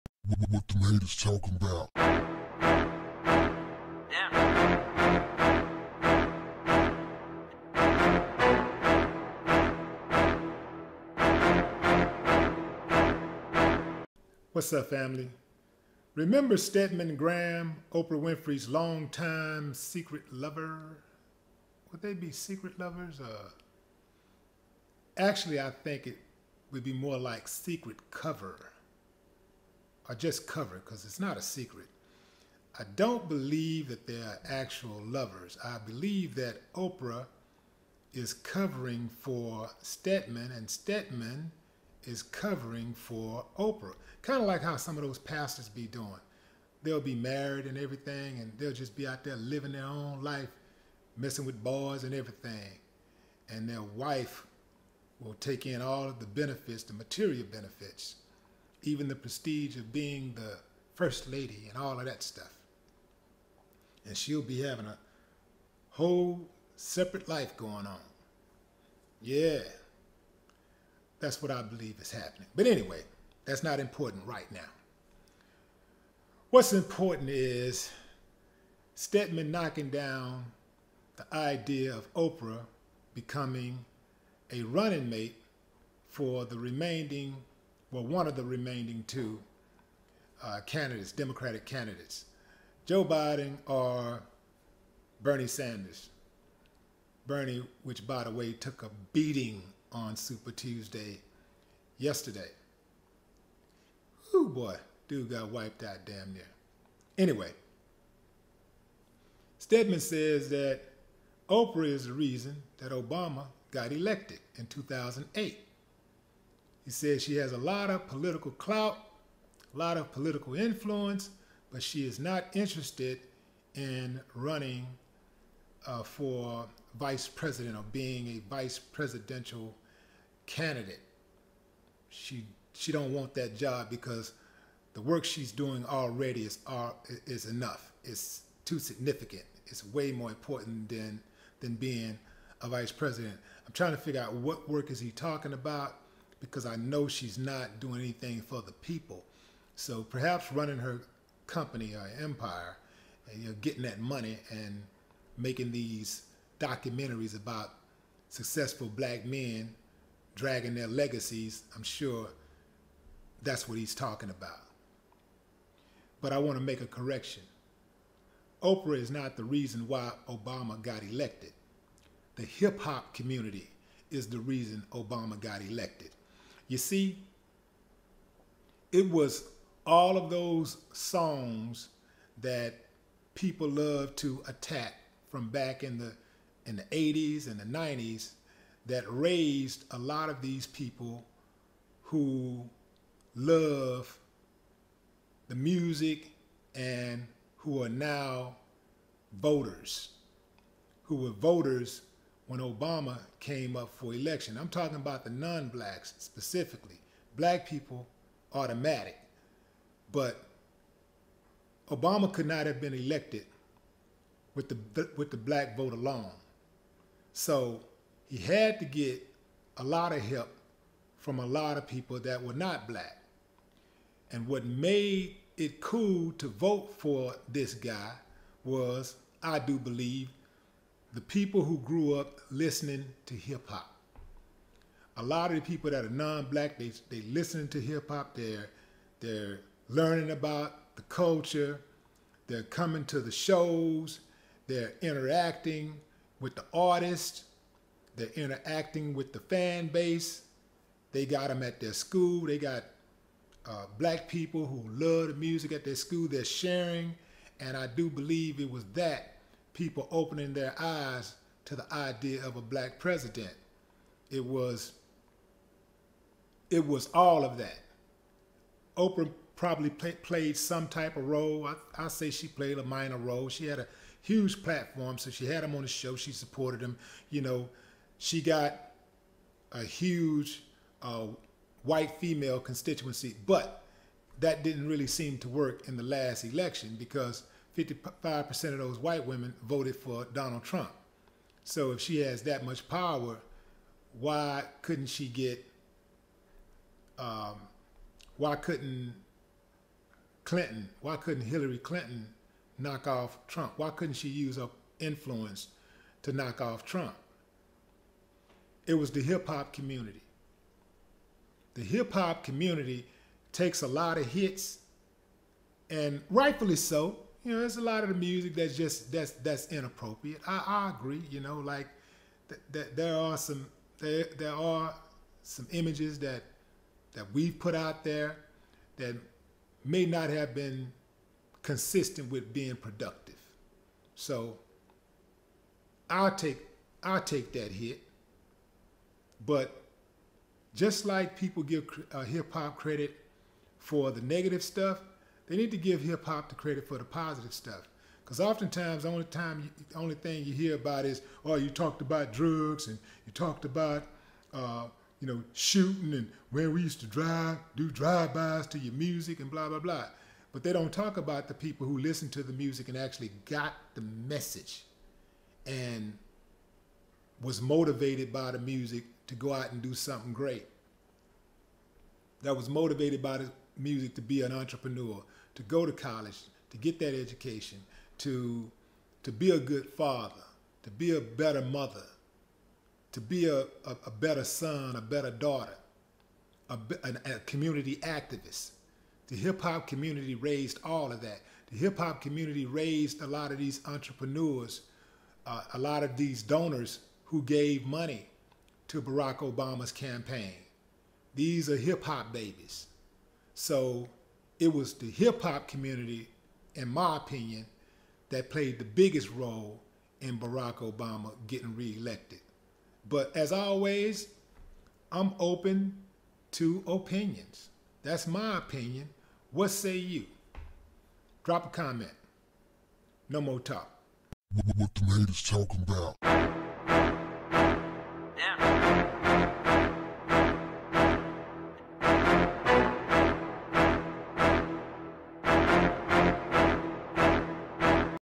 STEDMAN GRAHAM: OPRAH WON OBAMA THE WHITE HOUSE ...But She's No One's VP!!!
$WillieDLive

PLATFORMS